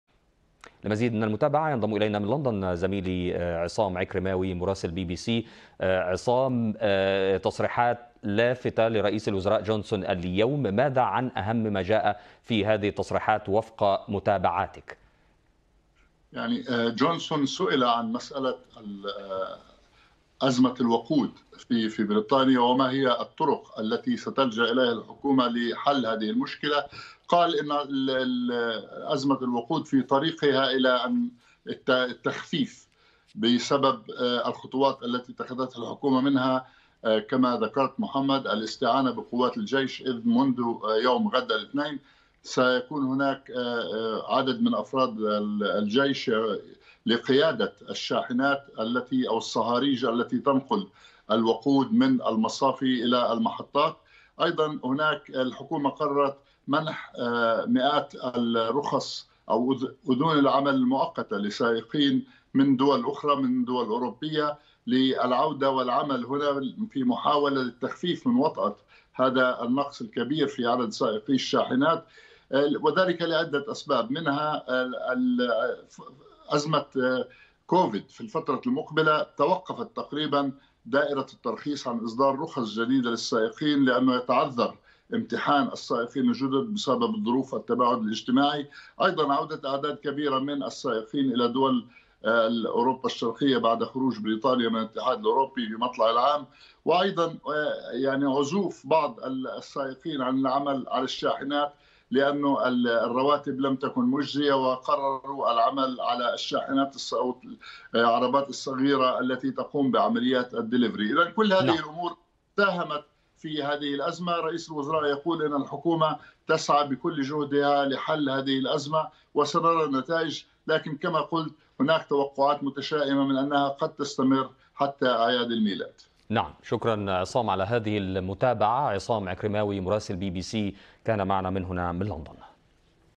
BBC Arabic TV 2021 استمرار ازمة الوقود في انجلترا وتوقعات متشائمة حول انتهائها
Johnson says fuel criss is abating in England